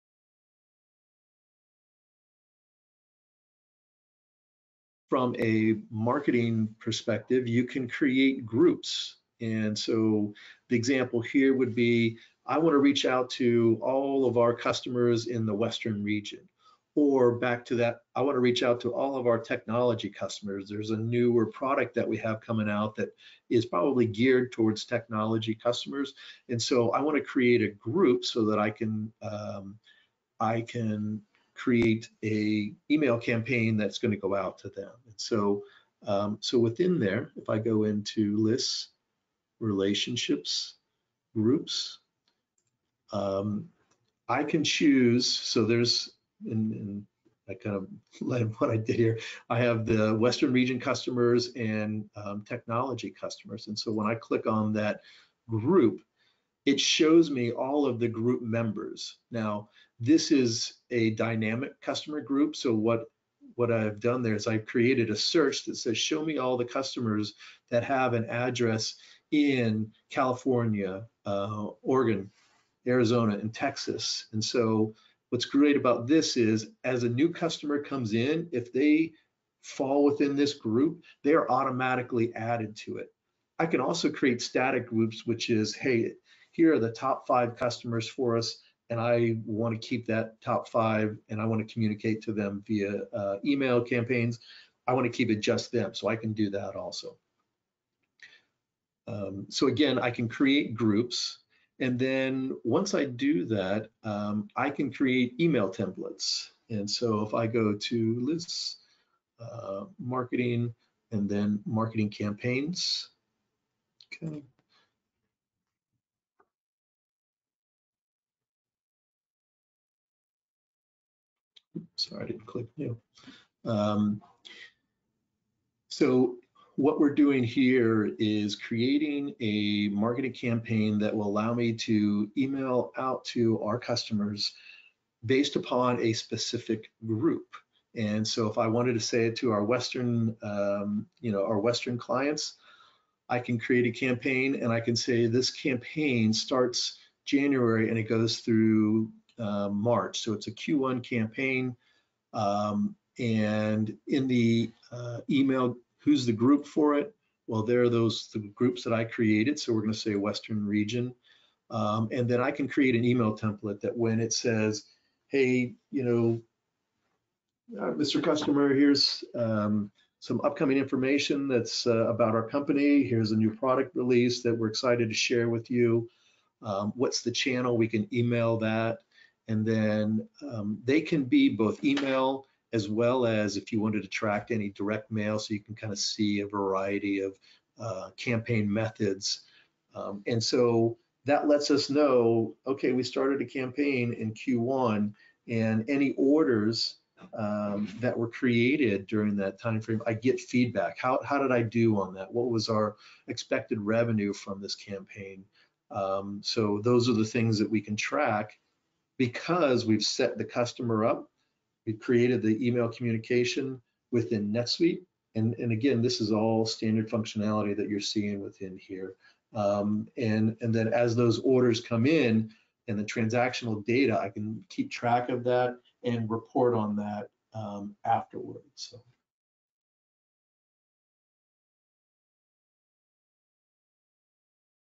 NetSuite Demo: Using Groups to Create Targeted Email Campaigns
Learn how utilize your contact groups within NetSuite to create targeted email campaigns to further built your relationships on a more personalized level.

As new contacts are added to your email list, understand how organize them  to further maintain your targeted email campaign.